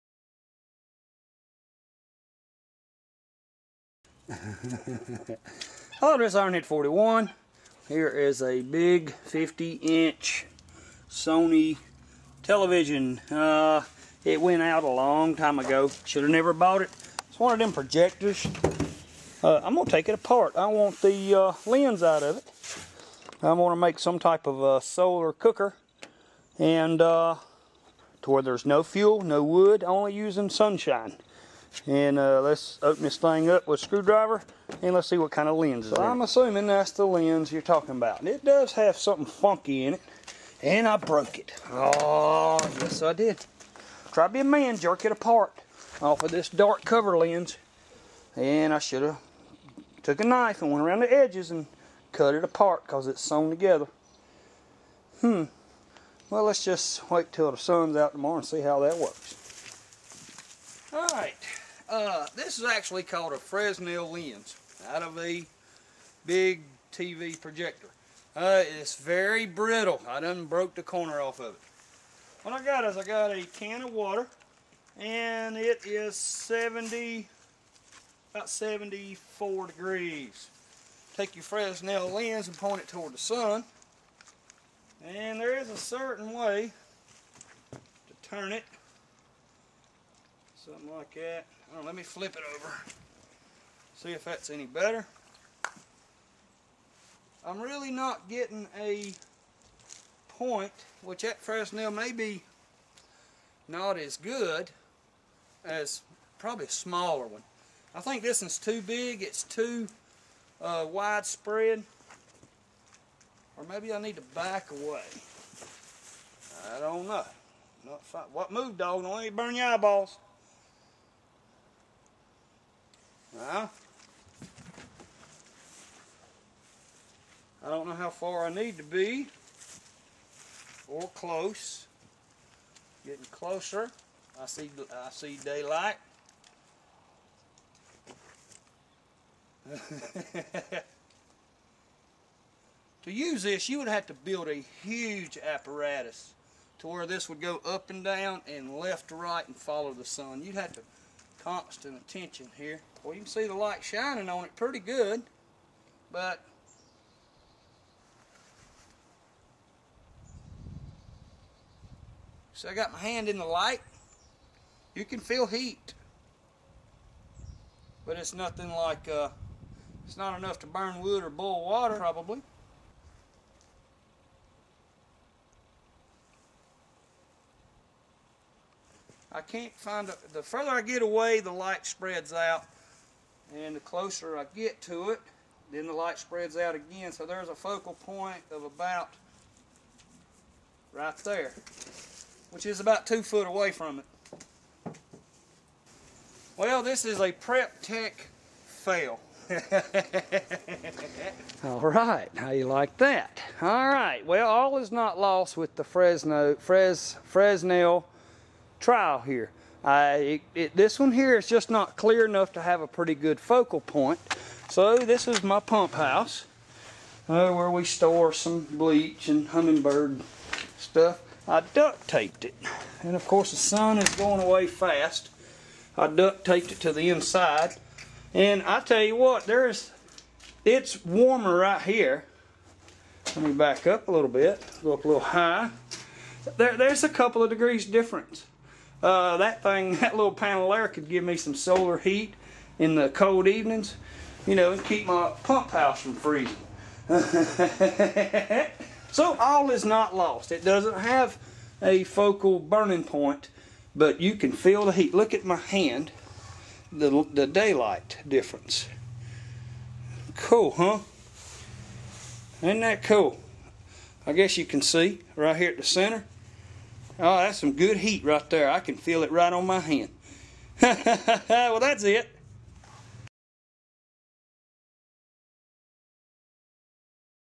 GoatHollow Prep Tech challenge fresnel solar fail
but all is not lost, still made it work for something